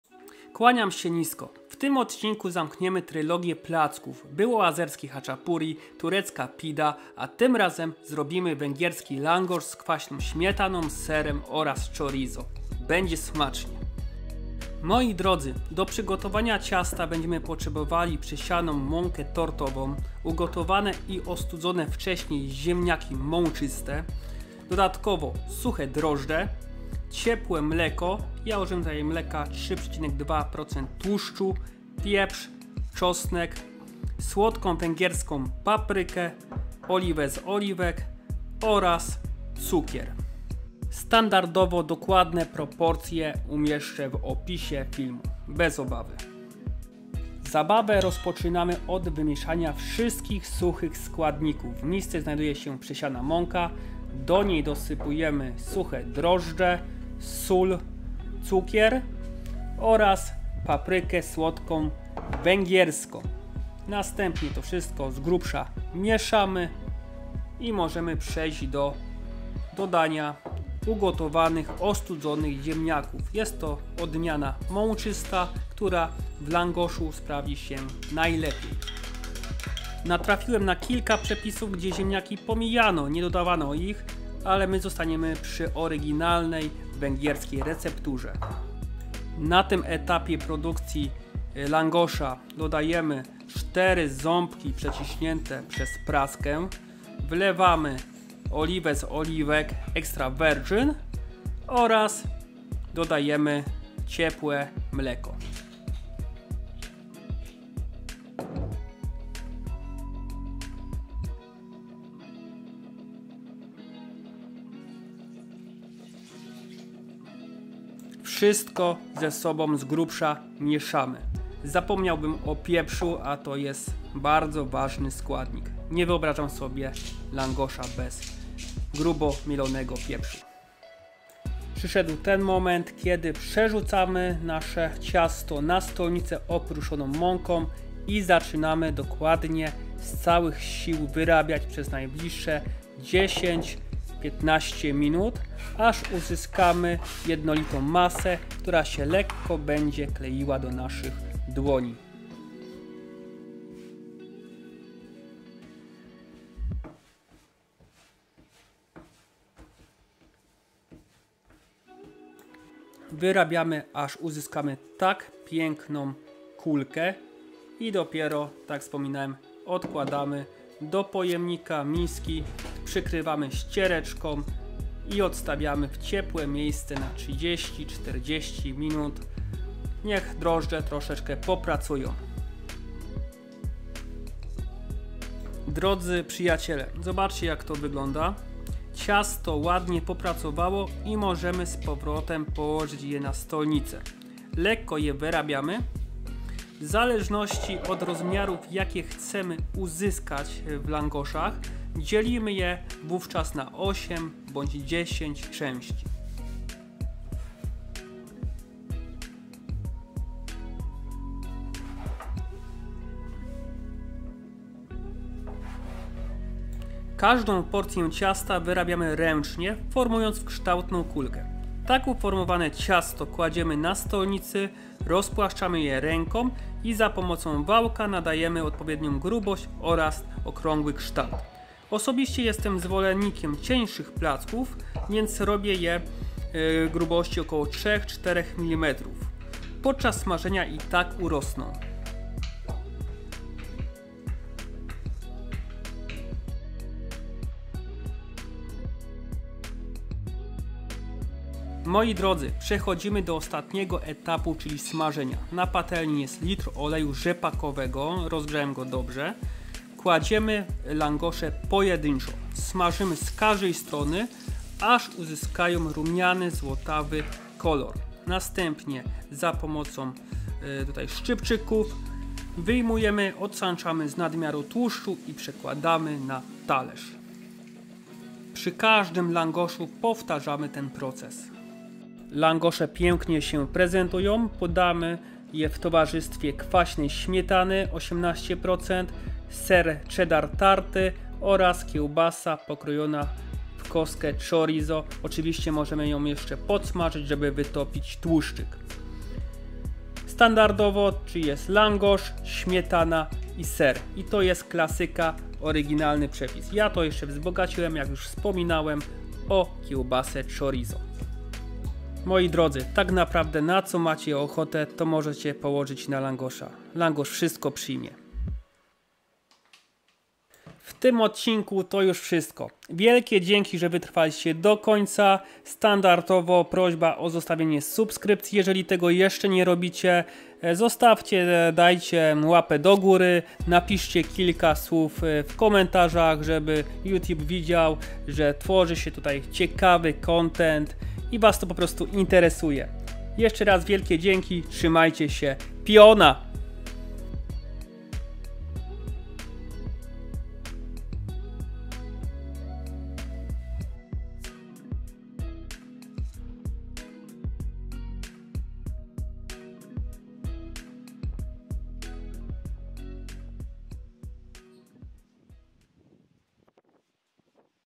LANGOSZ | węgierski placek z kwaśną śmietaną i serem
⚡️ Langosz węgierski street food⚡️

Składniki:

400 g mąki pszennej
200 g ugotowanych ziemniaków
7 g suchych drożdży
1 łyżeczka cukru
250 ml mleka
40 ml oliwy
1 łyżeczka papryki węgierskiej
4 ząbki czosnku
sól

Dodatki:
kwaśna śmietana
ser
ewentualnie kiełbasa chorizo